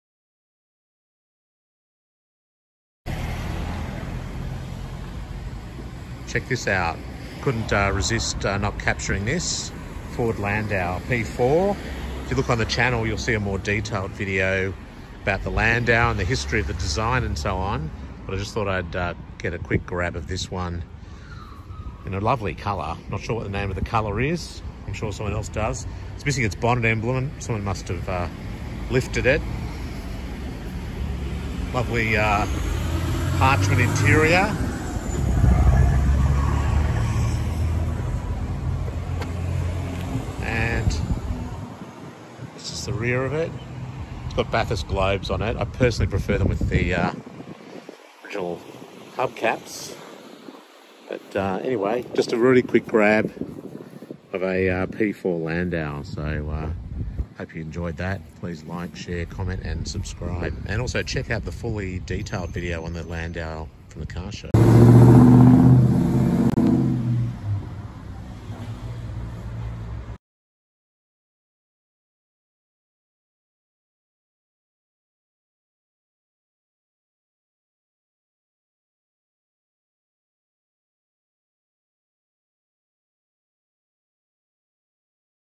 Super Rare Lime Frost P4 Ford Landau
I spotted this super rare Lime Frost Ford Landau today so wanted to share it. These Landaus were Ford Australia’s version of a Thunderbird - a personal luxury car amalgamating the Falcon GT hardtop with the Ford LTD. Really cool and I hope you enjoy this rare classic. 👍🤓